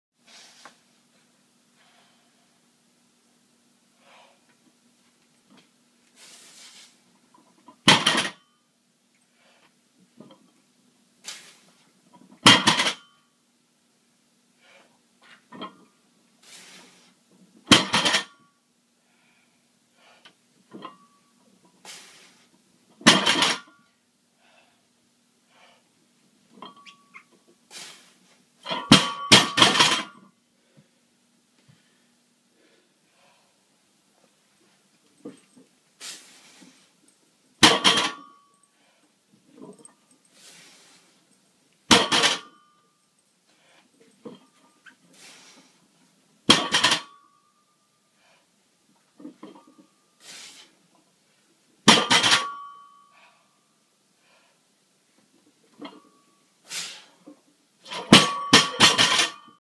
395 Lb Deadlift 3x5
The volume project continues. This is 70%. It was preceded by a front squat 3x3 at 70%. Had some knee pain so wore sleeves. Straps as I hook grip in competition.